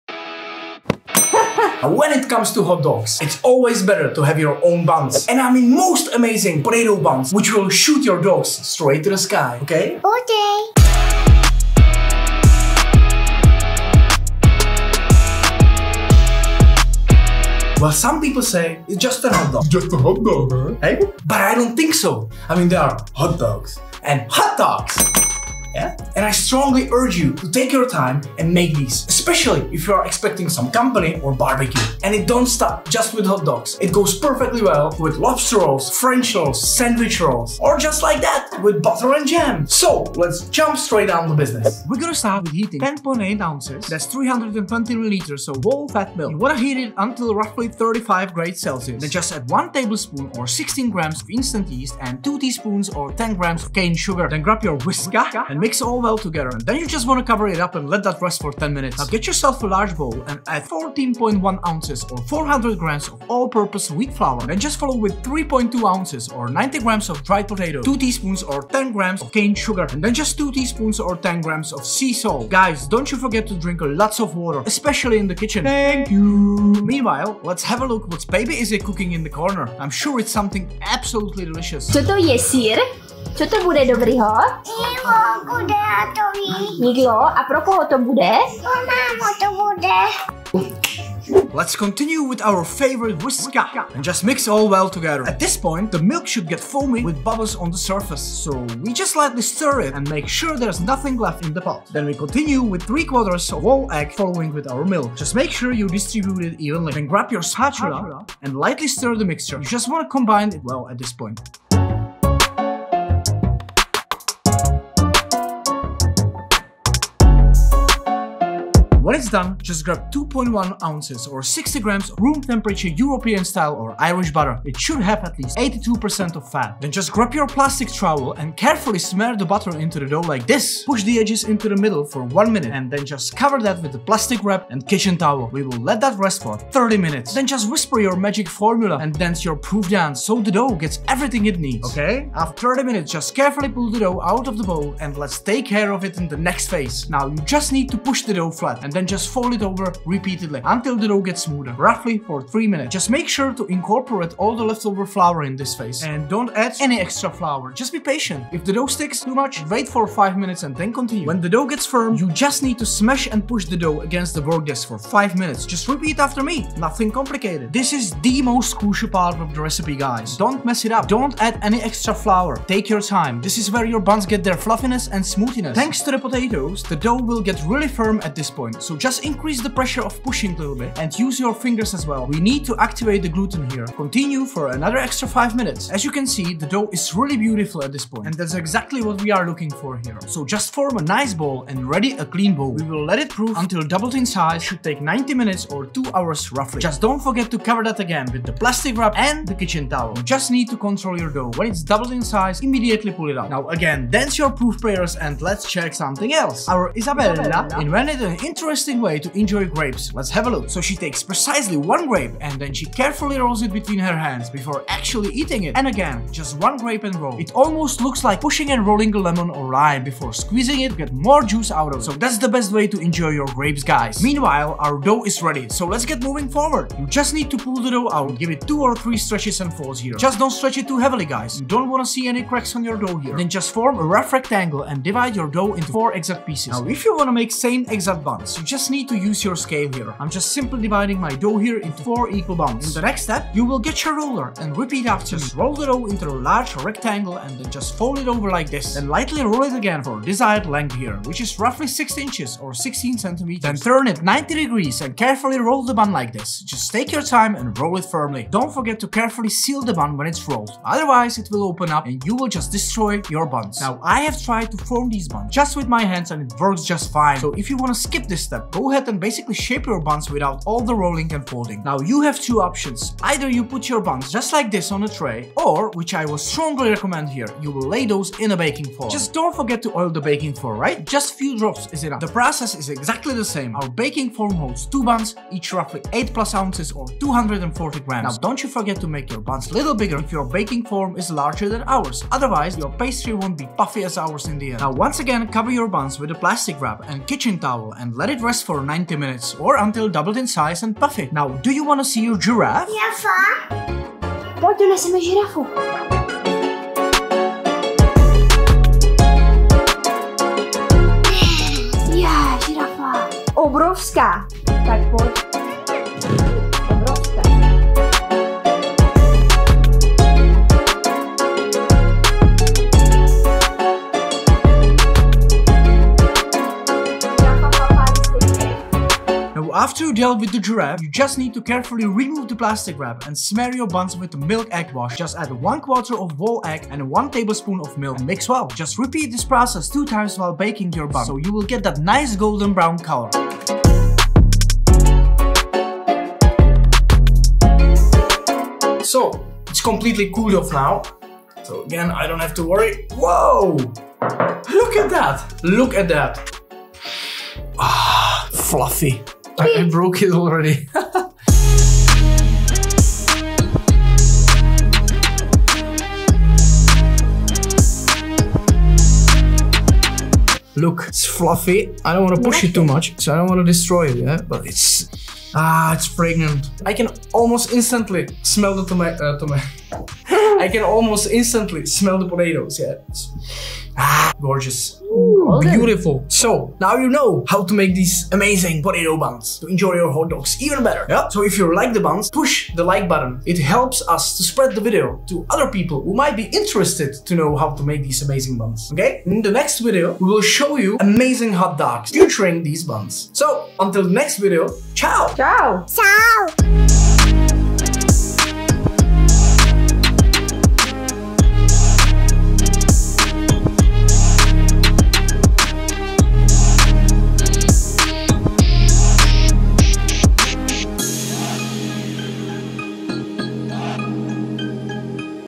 HOMEMADE Hot Dog Buns 🌭❤️ QUICK and EASY | From Scratch
Crispy and Fluffy, tastes like potatoes with butter!

Once you have tried these scrumptious potato Hot Dog Buns, you will never ever want to buy em again from store! Its Fresh, Fluffy and Smoooooth!

TIMESTAMPS

0:00 intro
0:17 lets talk about potato hot dog buns
0:41 potato buns / rolls
3:26 special way to eat grapes
3:44 building the rolls
5:03 mission giraffe
5:29 baking the buns
5:48  its finished!
6:10 checking  the buns + outro

INGREDIENTS  4 BUNS

14.1 Oz. / 400g of wheat all purpose flour
3.2 Oz. / 90g of dried (instant) potatoes
10.8 Oz. / 320ml of whole fat milk + 1 tablespoon for egg-wash
2.1 Oz. / 60g of European style / Irish Butter (82% fat)
1 whole large egg
4 teaspoons / 20g of cane sugar
2 teaspoons / 10g of sea salt / kosher salt
1 tablespoon / 14g of instant dry yeast